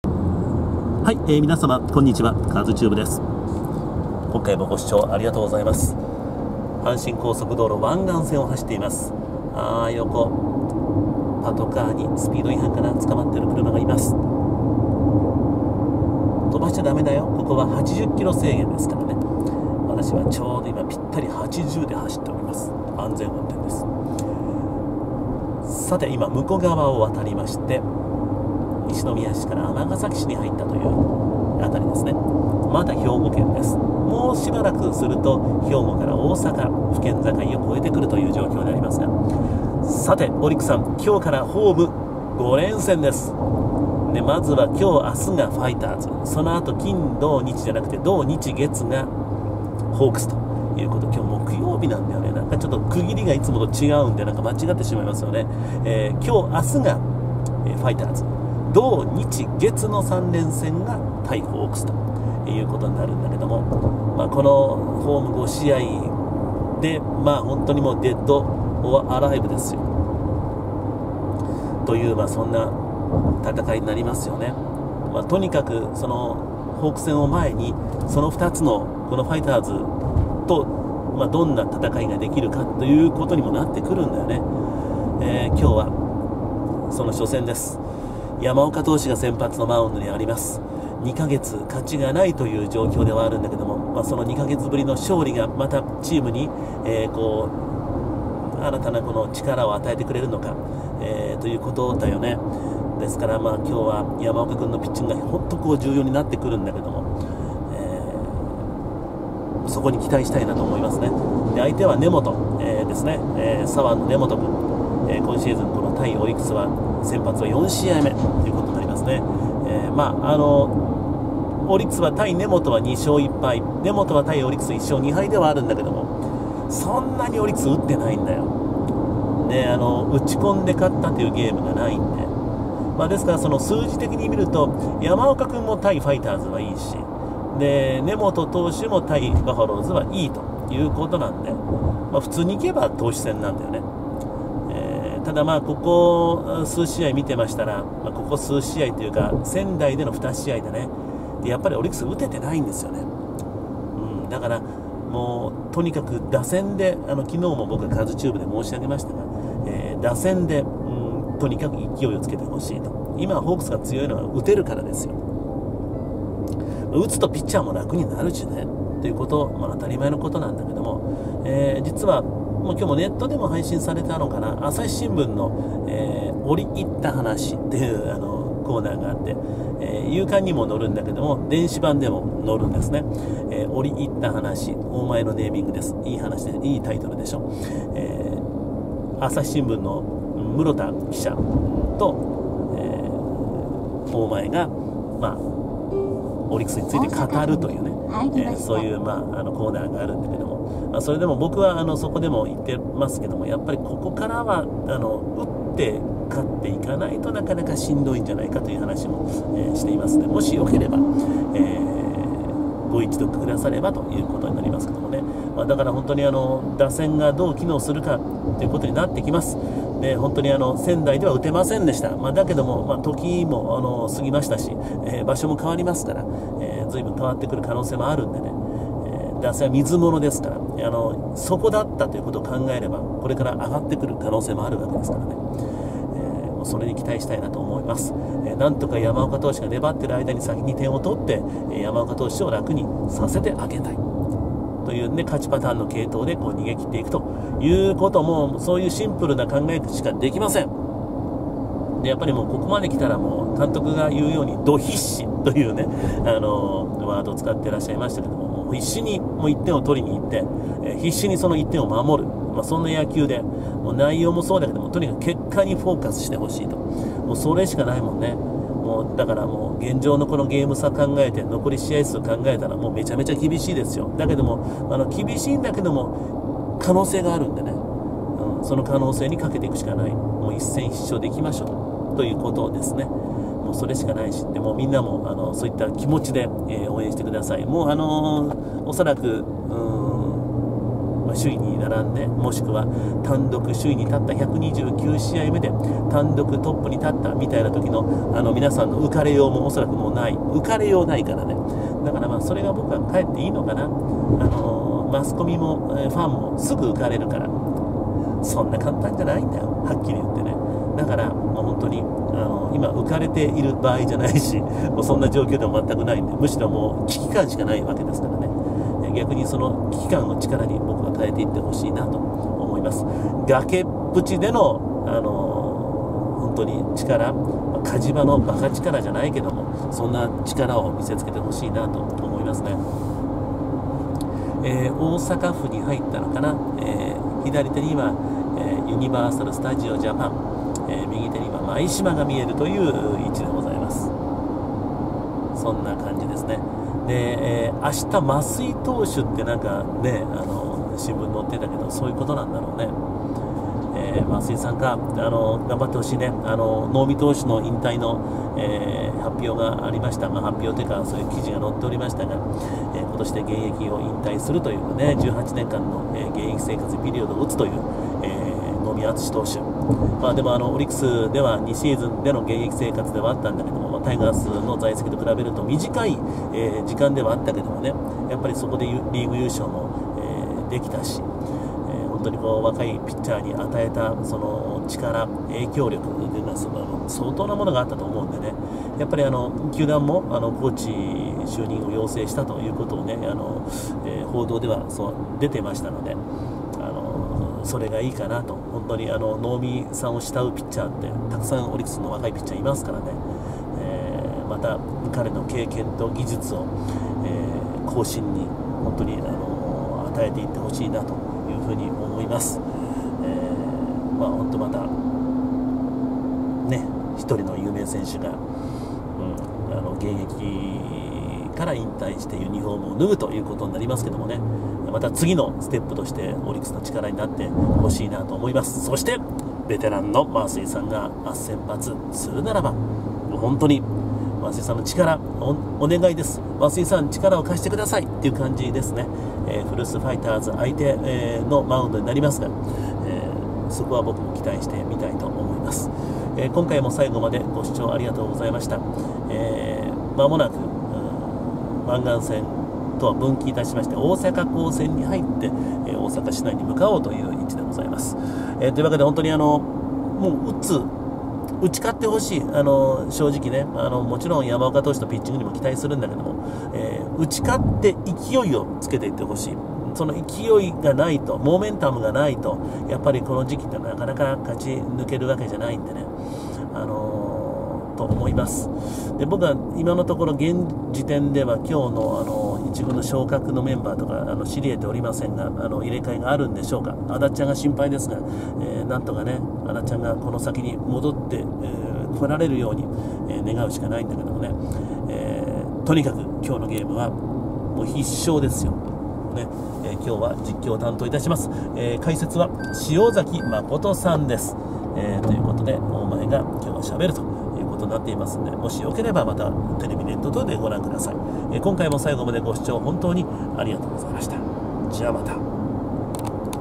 KazuTube！オリックスさん、今日からホーム5連戦！土曜日からのホークス戦を前に、挑戦権を保持できるかどうか、試される対ファイターズ２連戦。その初戦！
絶対に負けられない試合です！